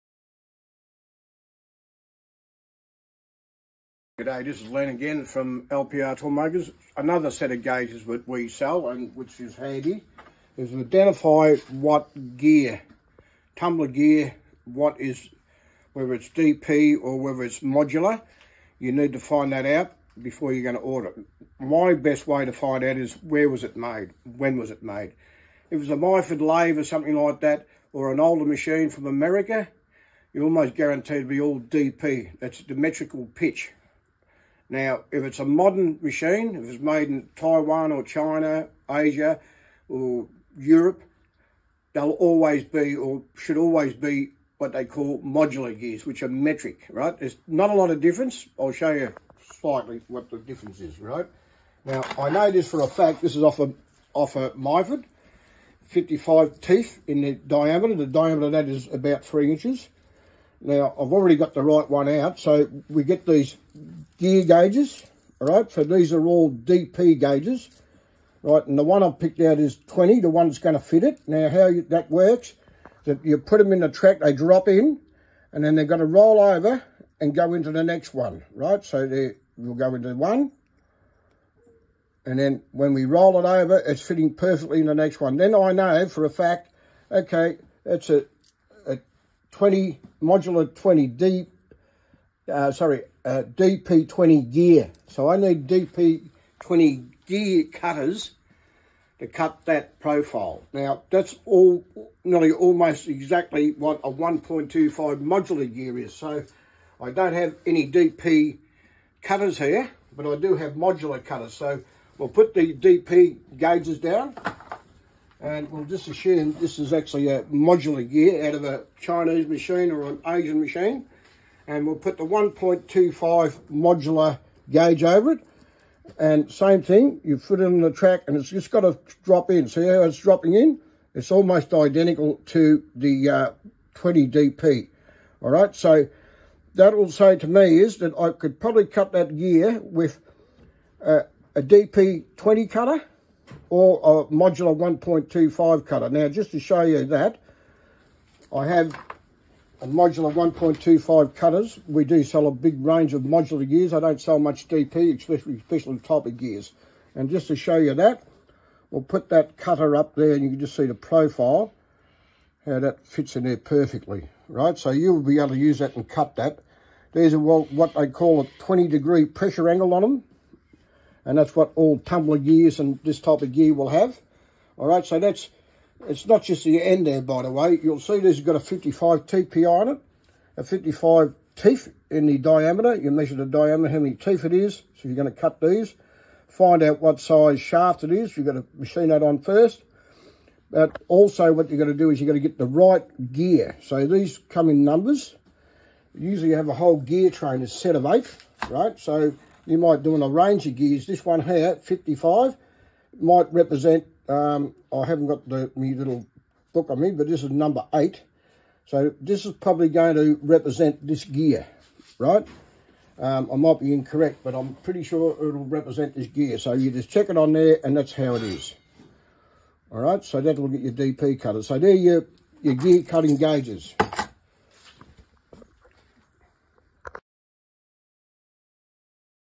D.P Gear tooth gauge set
This is an educational video produced by LPR Toolmakers

14 leaves, 22 sizes - 6 to 80 DP
20 DEGREE PRESSURE ANGLE

 Extremely hard to source set of D.P Gear gauges range from 6 to 80 D.P Imperial gear range they include the following profiles

INVOLUTE GEAR TOOTH GAGE, 14 leafs

Features:

With this Involute Gear Tooth Gage, it is easy to determine the diametrical pitch of involute gears from 6—80 and 2—5 in inch.
Pressure angle = 20º.
This gauge is made of hardened & tempered stainless steel material 0.020” thickness. This is a very handy tool used to measure the Gear Diametrical Pitch for 20° pressure angle (Tooth profile - Involute type) This gauge has 14 Blades duly fine edge profiled, Numbered pitch sizes are mentioned on each blade. it is very useful gauge for all gear manufacturers for inspection purpose. it is packed in a plastic box with gear data sheet (DP, CP, Tooth Thickness Data) provided for instant reference.

Range: 6 to 80 DP. (22 Pitches)